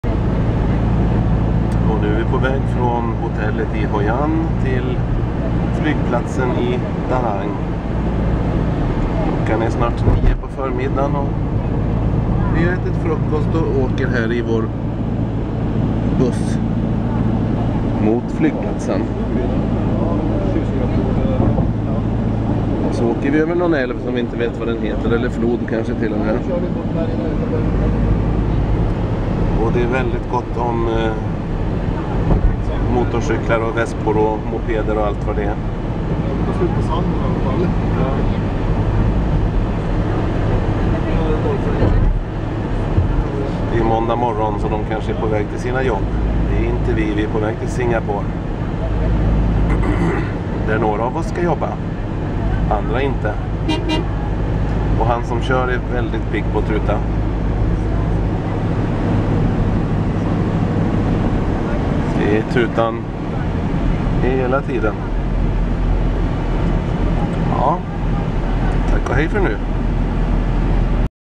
Mot flygplatsen i Da Nang Film 1
Den 1 april 2013 lämnade vi Hoi An i Vietnam för att via flygplatsen i Da Nang flyga till Kuala Lumpur och därefter vidare till Singapore.